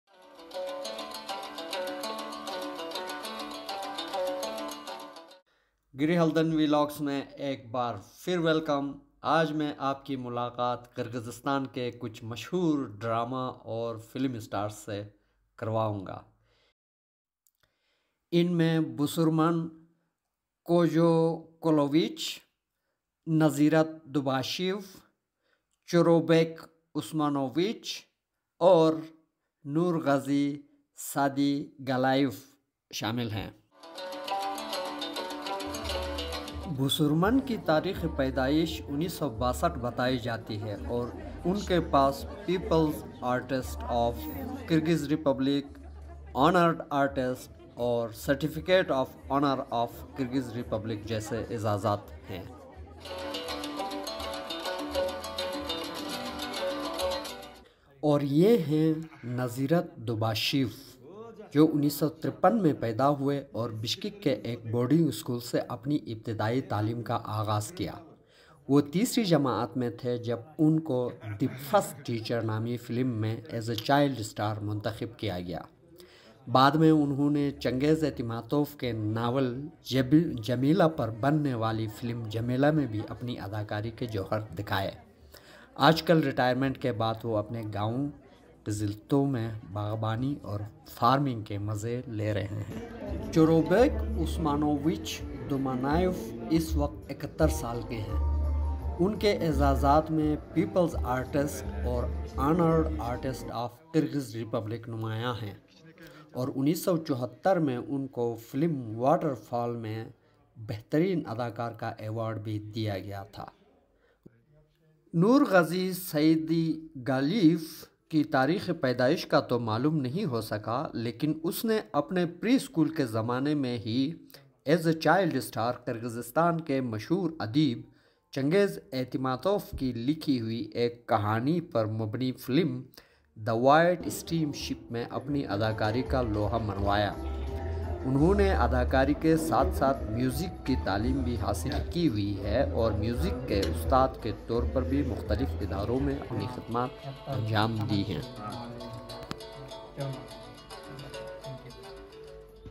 We Celebrated Film Actors of Kyrgyzstan | Film Actor of Kyrgyzstan |Chingiz Aitmatov
We had a great day with local figures of Kyrgyzstan who are well known for their creativity and work in Kyrgyz drama and theater and many of them got awarded for their services. this event was organized to commemorate the annual celebrations of Changiz Aitmatov the well-known Kyrgyz writer Changiz Aitmatov. These personalities played in the kyrgyz drama and films as child stars too. They were sharing their life experiences of the professional life and life of Chingiz Aitmatov Celebration 2023